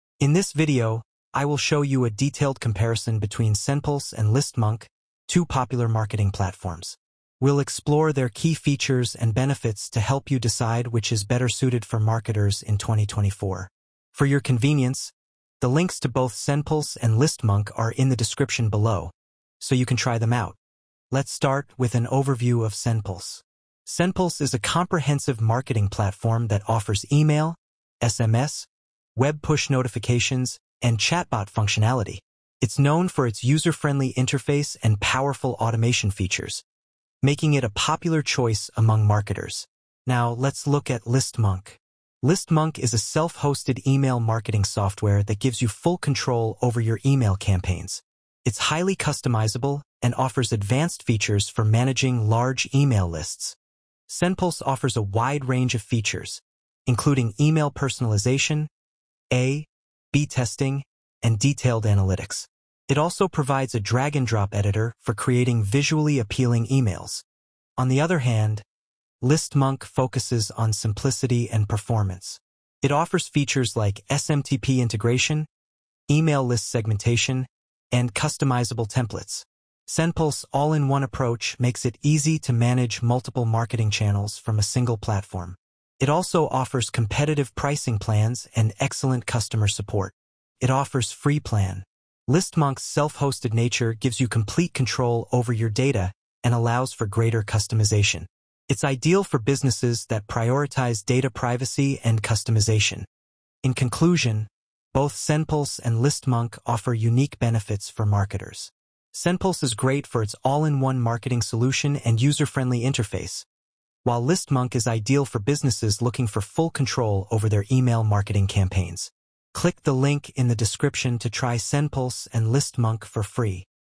SendPulse vs Listmonk - Features Comparison - Which is better for marketers 2025
SendPulse vs Listmonk - Features Comparison - Which is better for marketers 2024

In this comparison, we're looking at SendPulse and Listmonk to determine which is better for marketers in 2024. SendPulse is an email marketing platform known for its ease of use and comprehensive features, including a drag-and-drop editor, advanced segmentation, and SMS marketing. On the other hand, Listmonk is a self-hosted email marketing platform that offers similar features, including automation, personalization, and analytics.